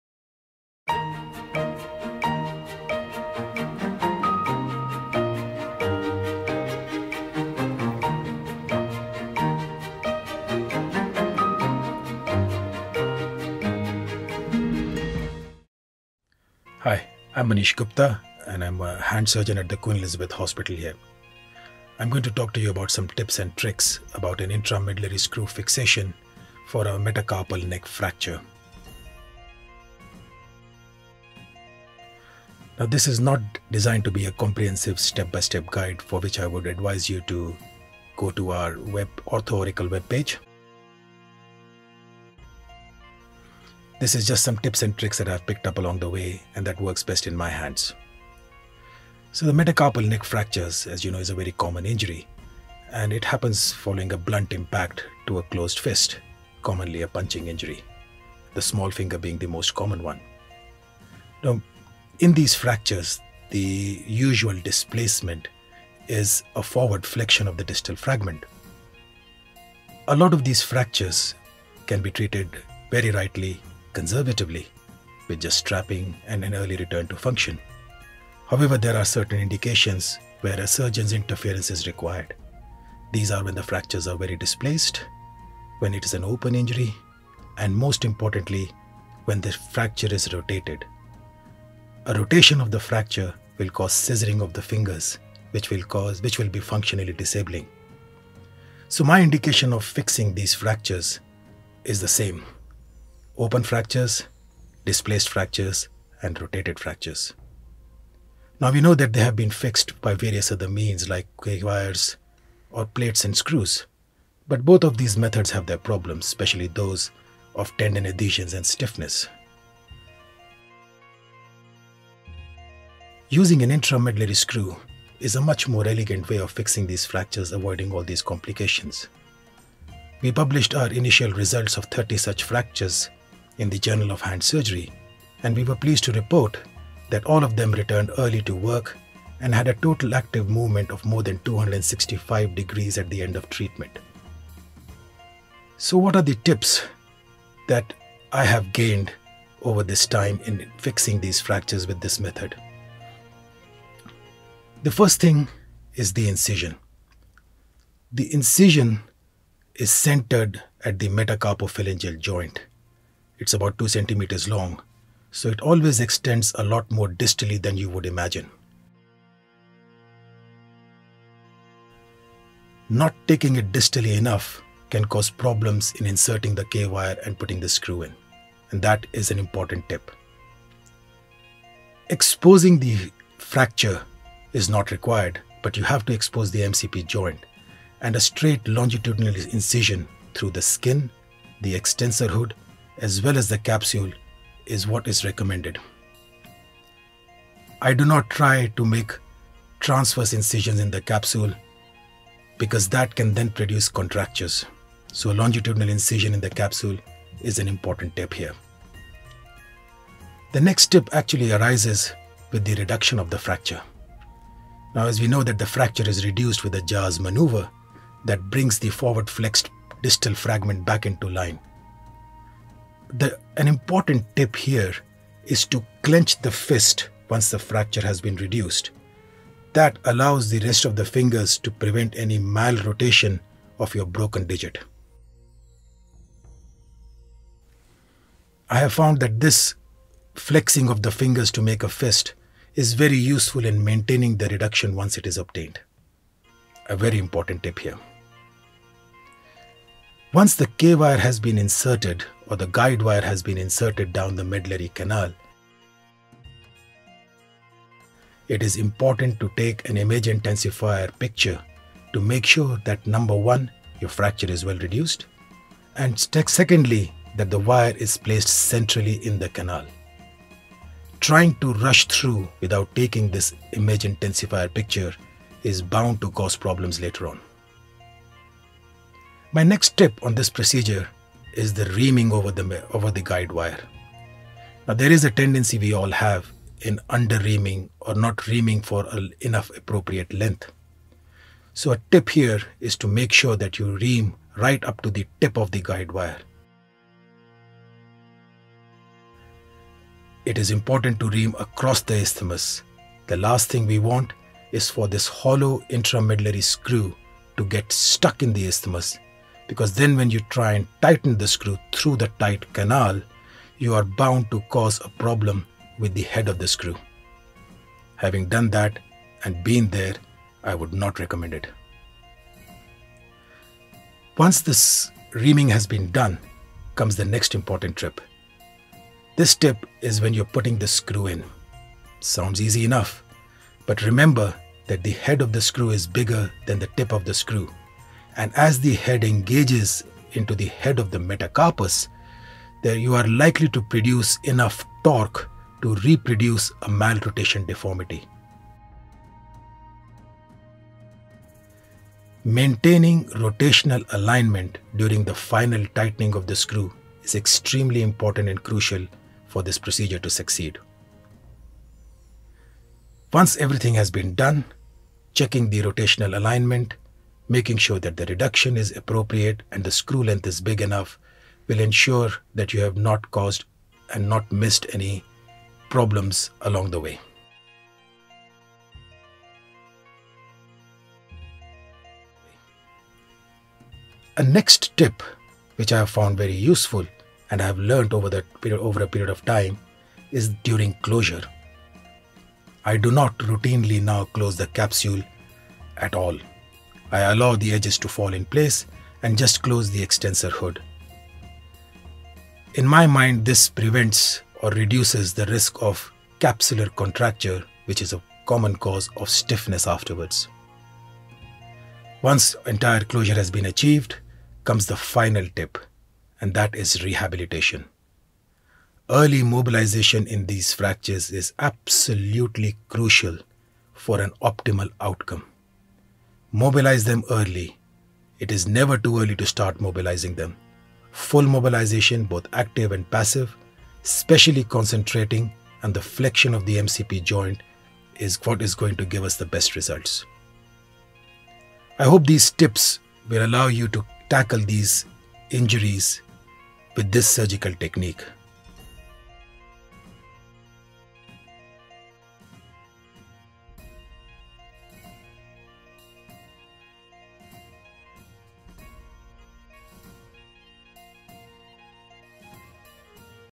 5th Metacarpal IM fixation
Manish Gupta  explains some of the key things to consider when using this useful technique to reduce and stabilise 5th metacarpal fractures.

This short video supplements  Manish Guptas comprehensive OrthOracle surgical technique: Intramedullary fixation of metacarpal fracture using the Acutrak compression screw which is one of a number of techniques detailed on the OrthOracle platform to surgically manage metacarpal fractures